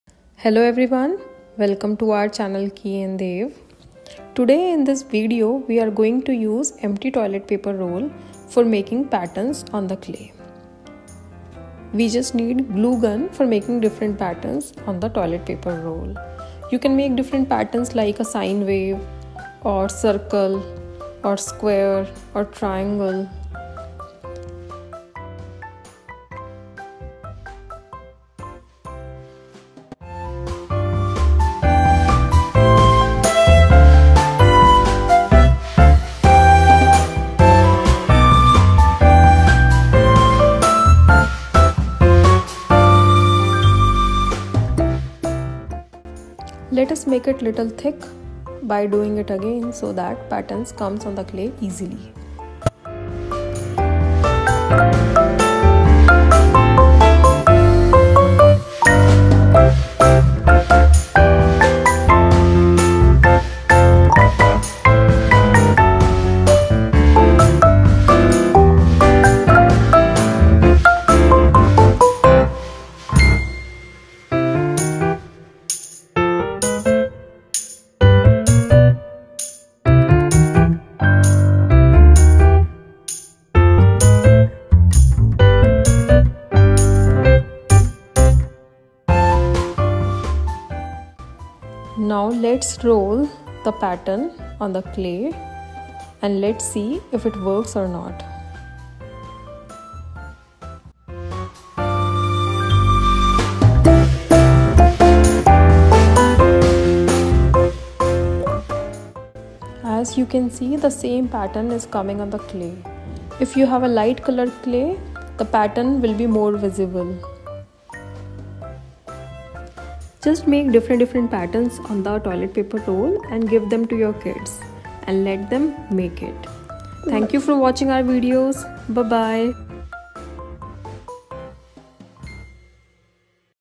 Use of Empty Toilet Paper Roll / Toilet Paper Roll Craft for Kids
Hello Everyone!!

In this activity, we use toilet paper roll for making patterns on the clay. We have made different patterns on the empty toilet paper roll by using glue gun. The patterns can be  dots, circle, wave, heart or some words also. Just roll the paper roll on clay and see the patterns on it.

This activity was really fun and you can also try it with your kids.

Let us know how you like it through your comments.